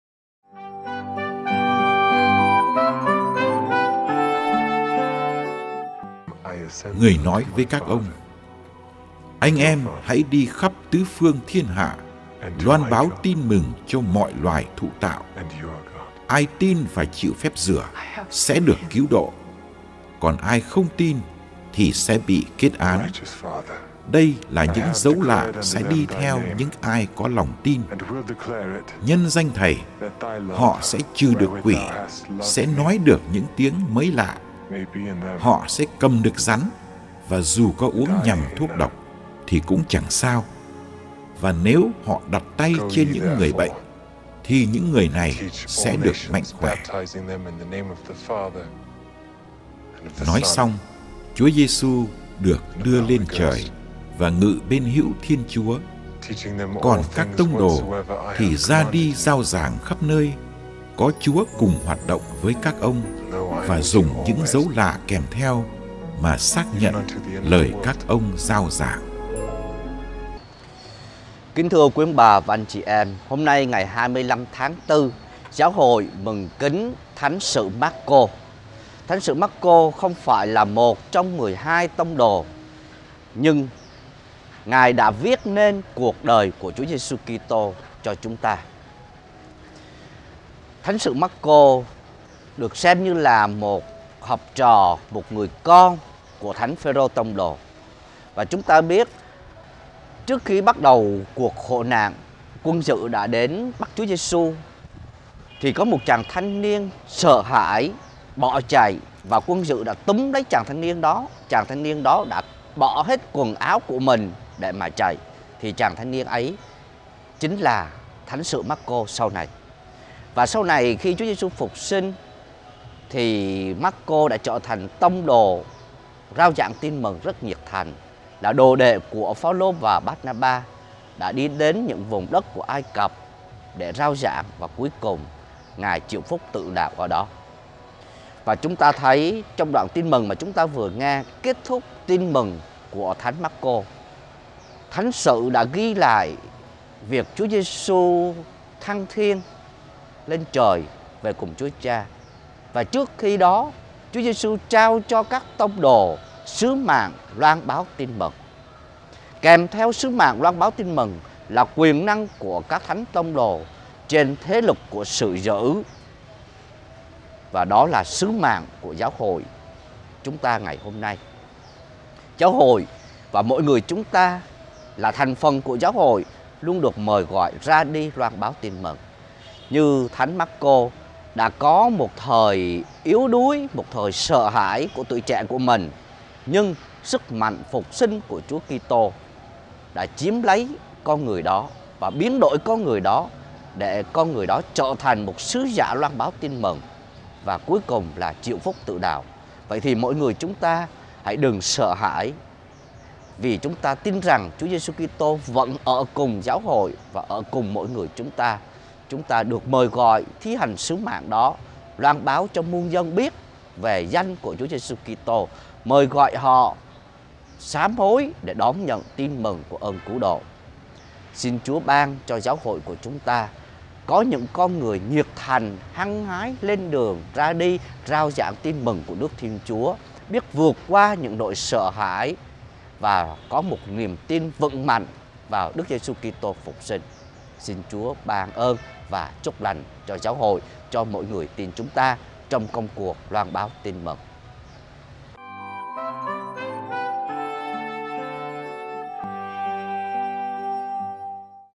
LHS Thứ Tư Tuần 4 Phục Sinh: VƯỢT QUA SỰ SỢ HÃI - Lm. Giuse Trương Hoàng Vũ, DCCT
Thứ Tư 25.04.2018
VƯỢT QUA SỰ SỢ HÃI
Linh mục Giuse Trương Hoàng Vũ, DCCT